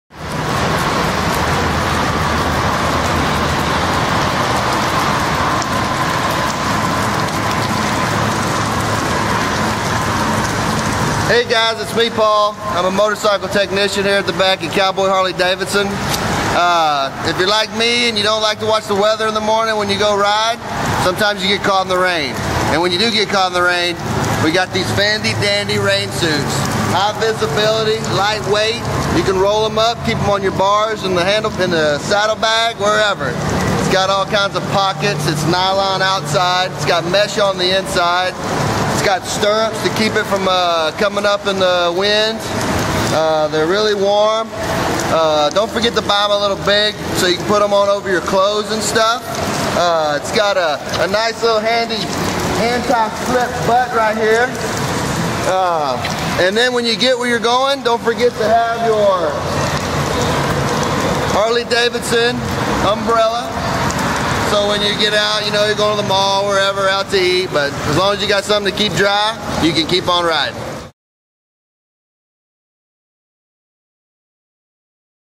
Harley-Davidson Hi-Vis rain Suit
Available now at Cowboy Harley-Davidson of Beaumont.
Lightweight waterproof high-visibility 100% nylon with mesh lining. Double storm-flap zippered front with hook-and-loop closures. Two zipper hand-warmer pockets and one inside chest pocket. Stretch fit hood reduces bulk to fit comfortably under helmet. Pant features 3/4 leg zipper and heat shields with extended protection on right leg. Rear-end anti-slip traction. Stirrups, removable suspenders and easy-access thigh pocket. 3M™ Scotchlite™ Reflective Material stripes and graphic for additional visibility. Measures 14" x 13" when packed.